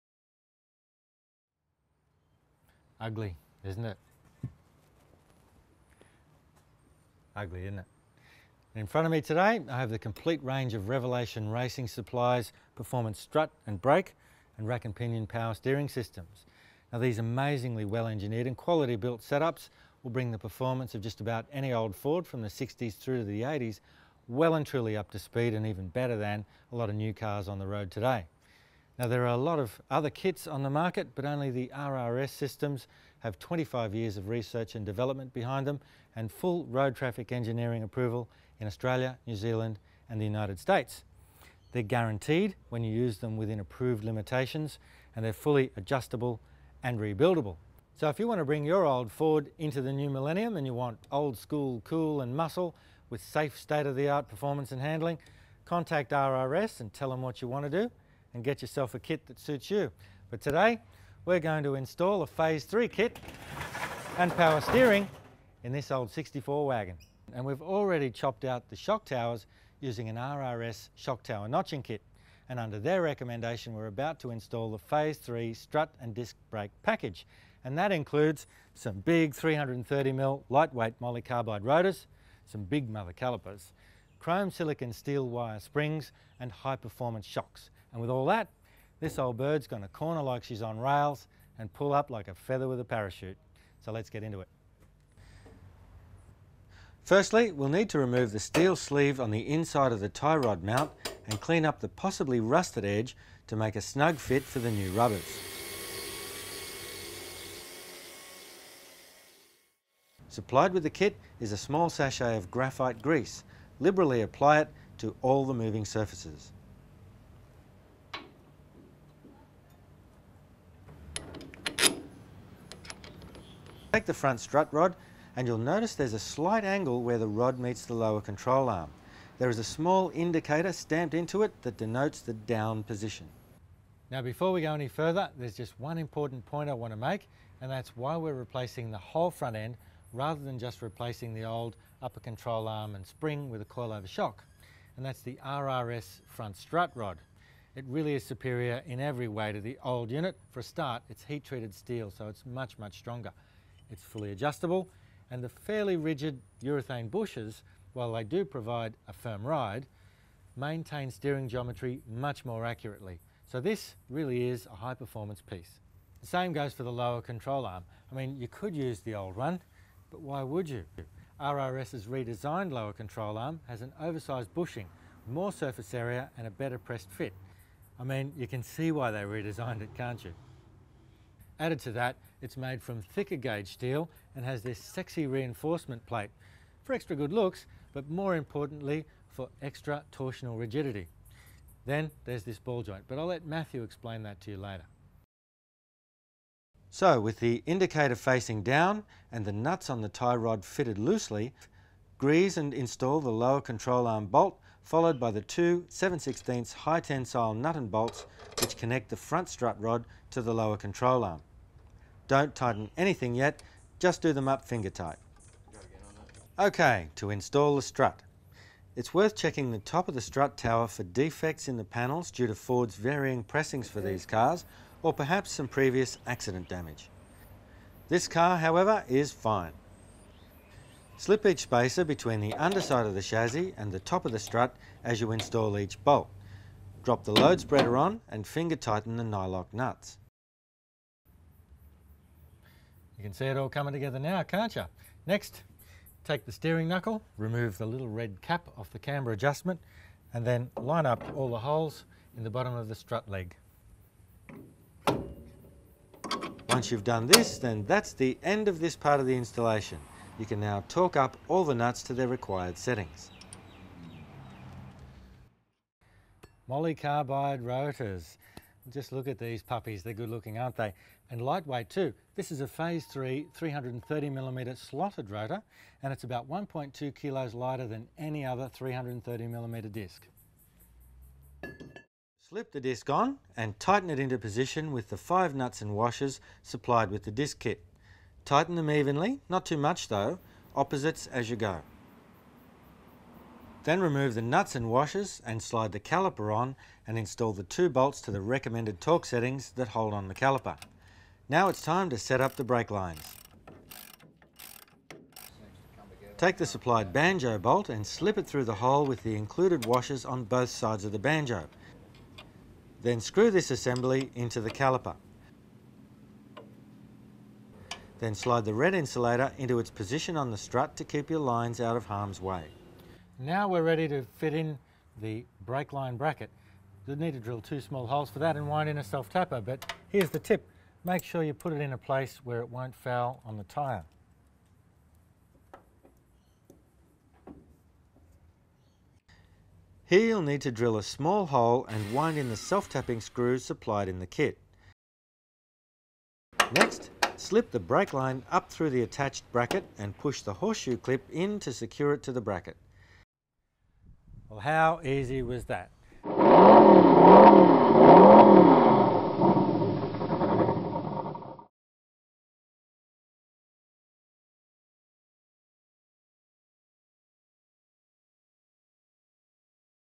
RRS front strut and disc installation....
A 6  minute video detailing the installation of the RRS phase 3 strut and disc brake package